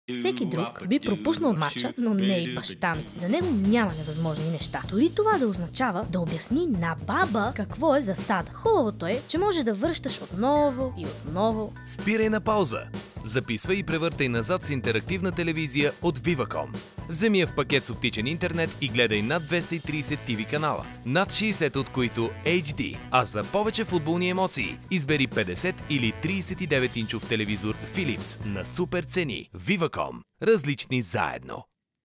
Вземи 50 или 39 инчов ТВ от VIVACOM за повече футболни емоции
Спирай на пауза, записвай и превъртай назад с интерактивна телевизия от VIVACOM! Вземи я в пакет с оптичен интернет и гледай над 230 ТВ канала, над 60 от които HD. А за повече футболни емоции, избери 50 или 39-инчов телевизор Philips на супер цени.